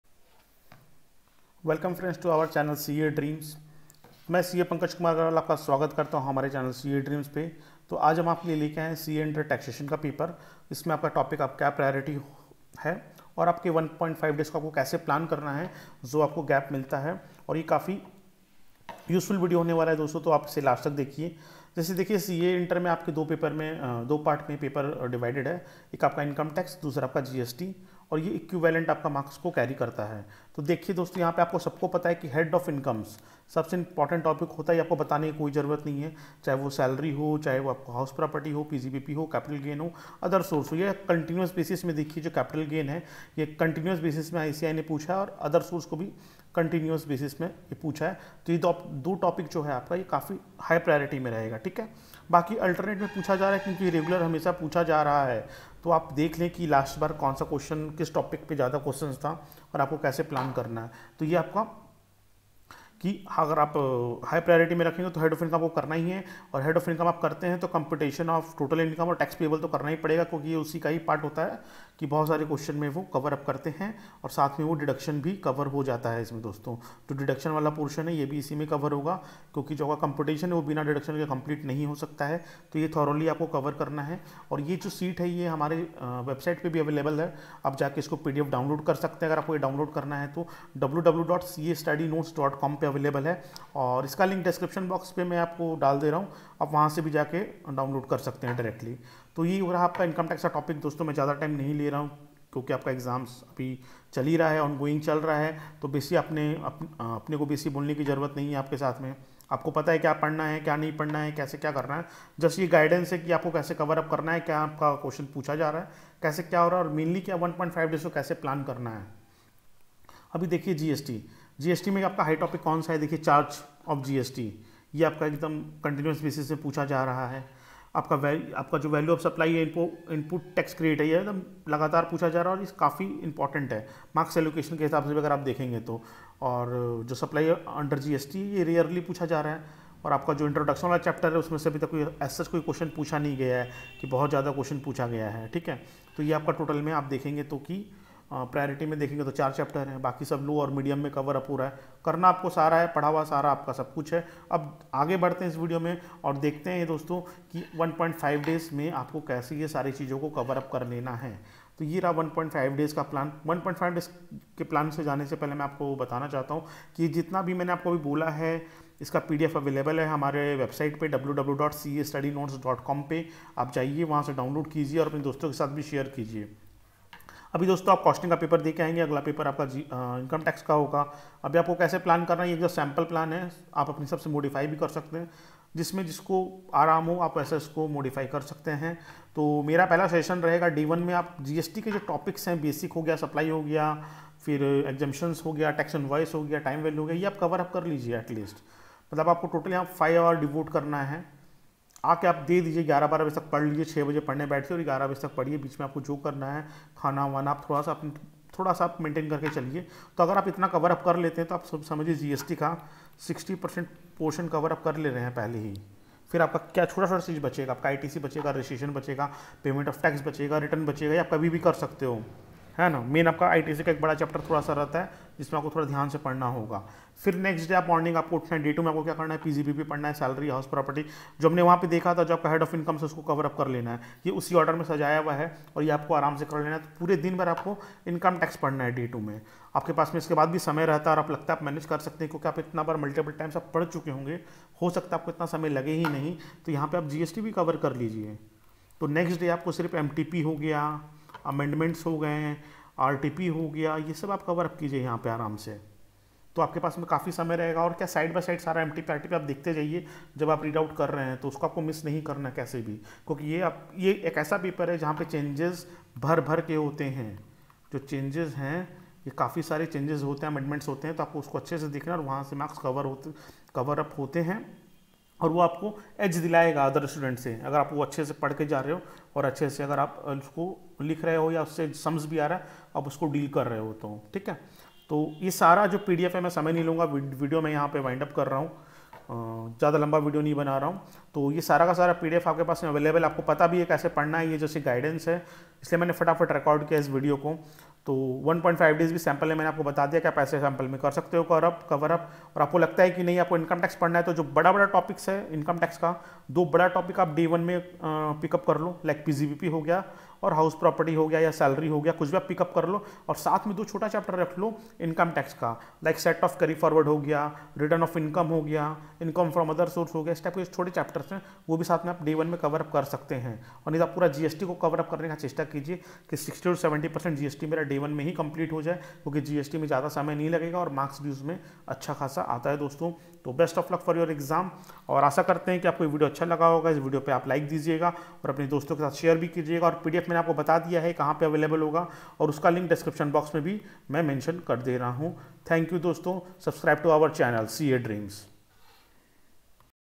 1.5 days Revision Strategy and Priority of Topics for CA Inter Paper - 4 "Taxation"
Hit Like to Video
Give Your Feedback in Comment.
" IT'S REALLY GIVES ME A MOTIVATION "